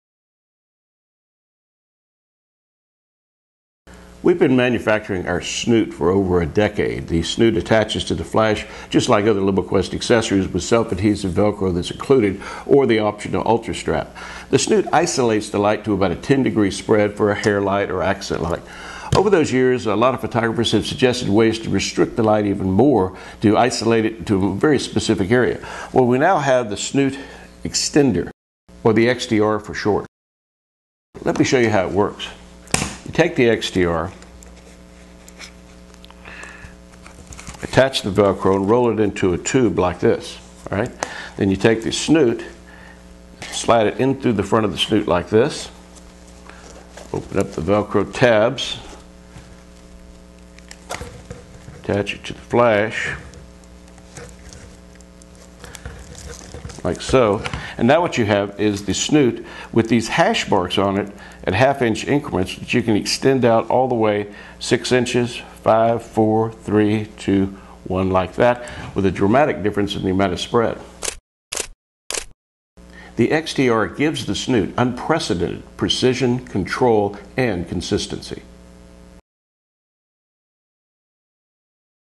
LumiQuest SnootXTR
Quest Couch demonstrates the Snoot XTR which isolates the light to a very specific area, approximately 10°. The telescoping extender enables the photographer to consistently isolate the light to an area of 5° or less. Automatic operation will be affected as illuminated area is limited; bracketing and/or testing is recommended.